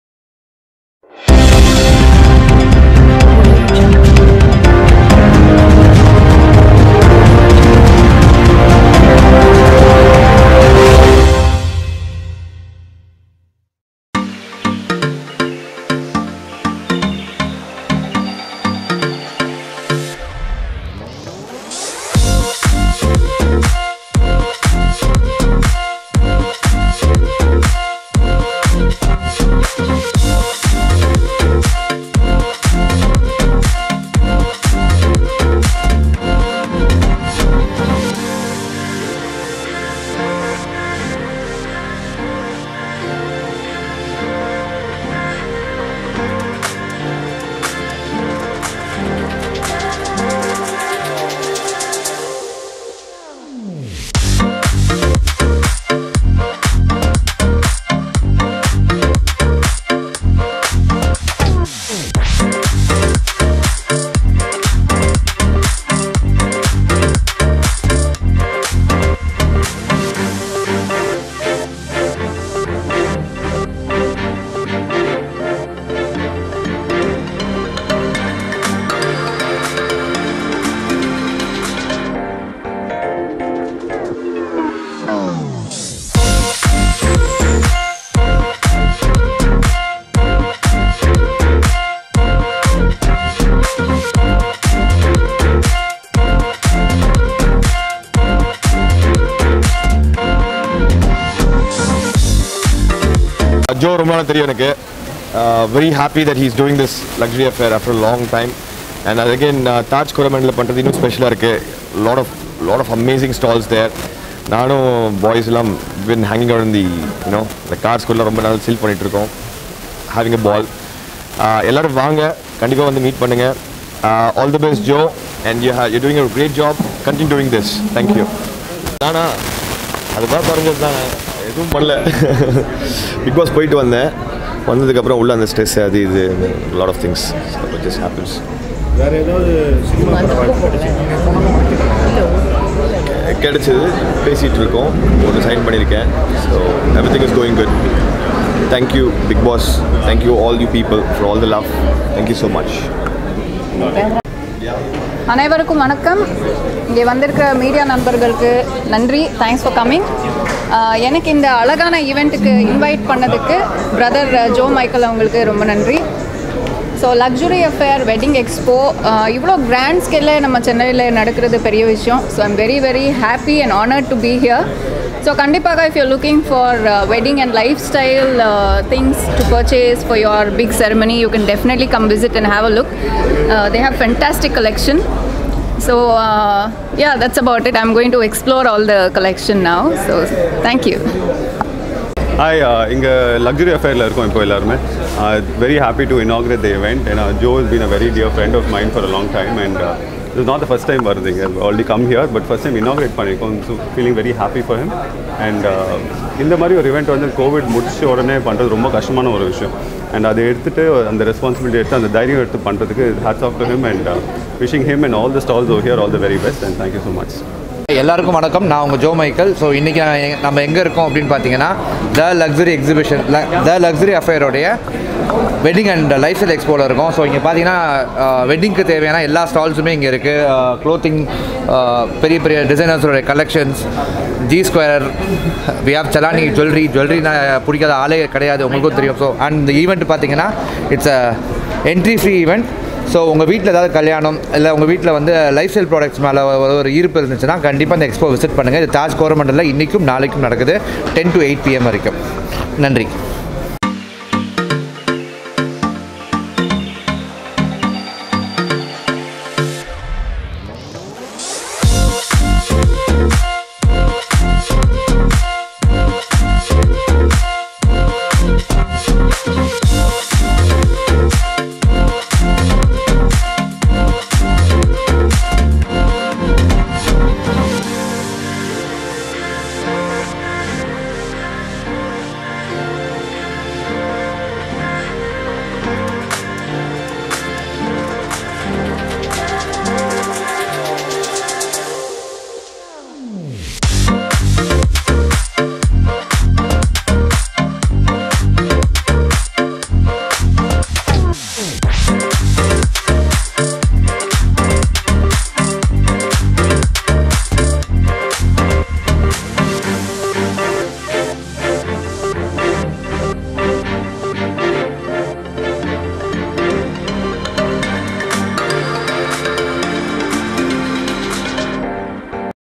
‘The Luxury Affair’ Chennai’s biggest and most awaited lifestyle event @Taj Coromandel
Chennai, May 15, 2022: Chennai’s biggest and most awaited lifestyle event, ‘The Luxury Affair’ was inaugurated by R.Purushothaman, Assistant Commissioner of Police, Avadi and Actors Sanam Shetty, Niroop Nandakumar and Abhinay Vaddi at Taj Coromandel, had a glittering array of luxury lifestyle products and the very best of wedding fineries. The two day event is a must visit on everyone’s social calendar and especially those who have a wedding coming up. Wedding & Lifestyle Expo took the center stage this year with major wedding vendors for clothes, jewelry, decorations, travel and everything wedding related were on exhibit.

In the grand event, ten exceptional people from Tamilnadu will be awarded the ‘Face of Tamilnadu’ Awards for their contribution to the society. The awardees include Padmasingh Isaac of Aachi Masala, Dr. Saranya Sree Sakthikumar, Director of Panimalar Group of Institutions, Advocate Kanimozhi Mathi for his legal role in Keezhadi Excavation, Social Activist Father Jegath Gasper Raj among others.

Luxury Affair will also host 'The Fashion Walk’ where leading fashion designers from South India like Sidney Sladen and Chaitanya Rao, reputable retail shops and jewelers will showcase their latest collections and designs. The Fashion Walk will be choreographed by acclaimed Choreographer Vignesh Chandrasekhar.

On 16th May, Mrs. Tamilnadu Awards are distributed to exceptional women entrepreneurs and achievers in 25 categories. Razzmatazz Wedding Awards are also given to prestigious wedding planners, vendors and pioneers in the wedding industry. The Luxury Affair Awards are issued to renowned fashion designers and fashion houses for their contribution.

For Further details:
Joe Michael, Razzmatazz Group: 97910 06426